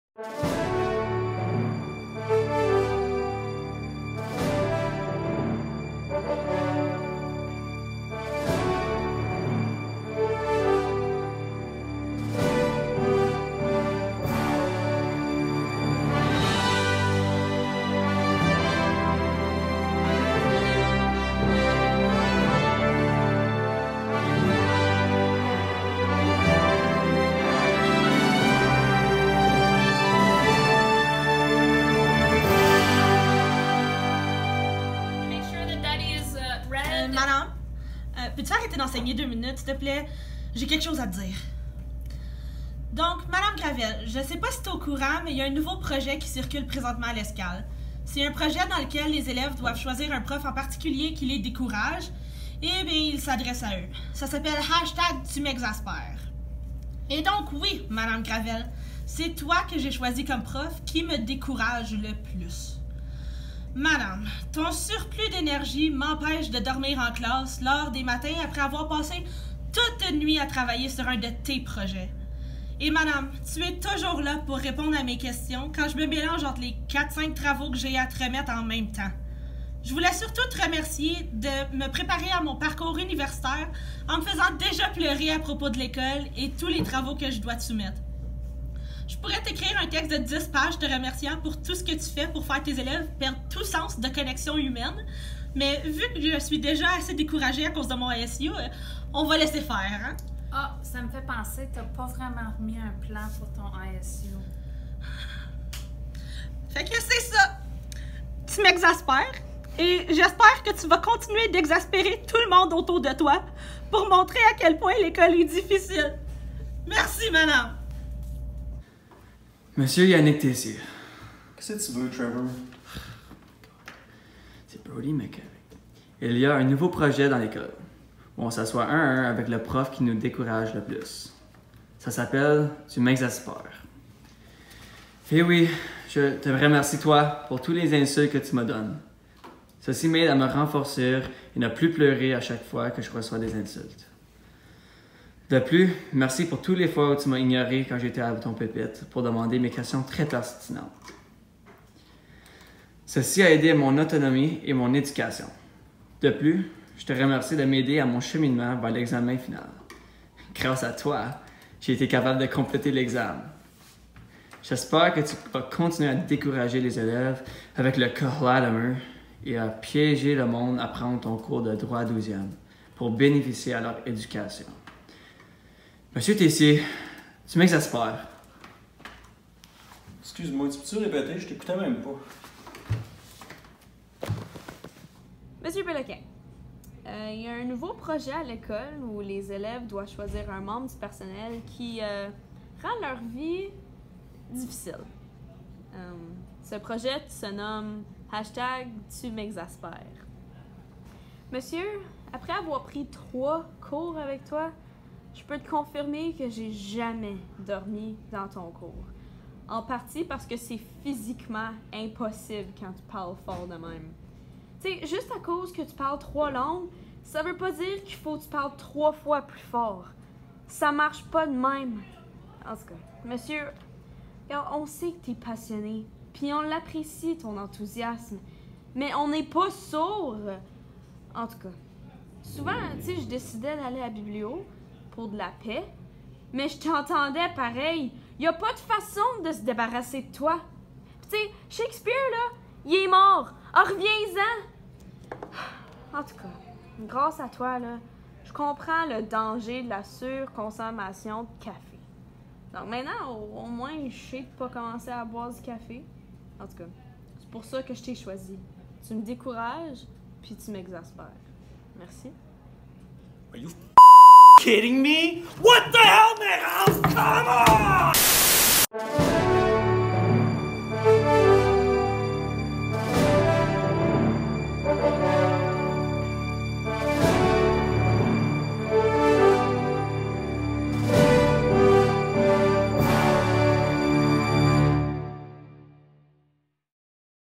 tumexasperes
Parodie du projet tuminspires présentée dans le cadre du Bye Bye 2018 à l'École secondaire catholique L'Escale.